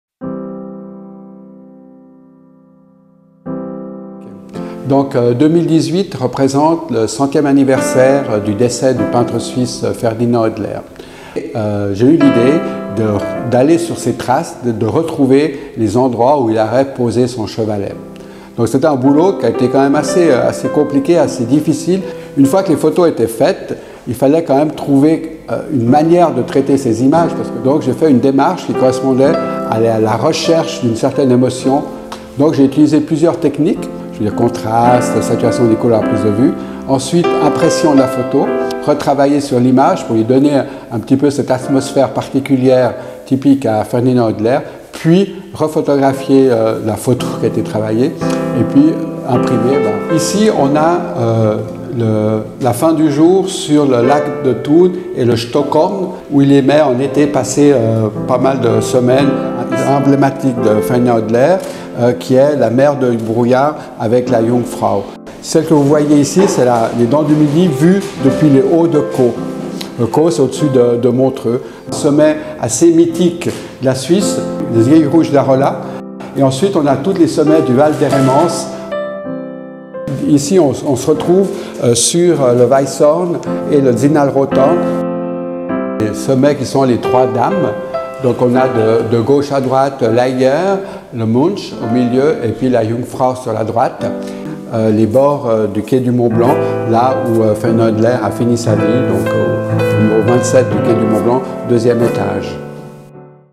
Charly Schwarz: Tras las huellas de Ferdinand Hodler
El fotógrafo suizo cubano Charly Schwarz parte sobre las huellas del artista suizo Ferdinand Hodler a través de una recreación fotográfica original. Toma las imágenes en el mismo lugar donde el pintor helvético plantó su caballete par realizar sus obras. Homenaje en el centenario de la muerte de Hodler.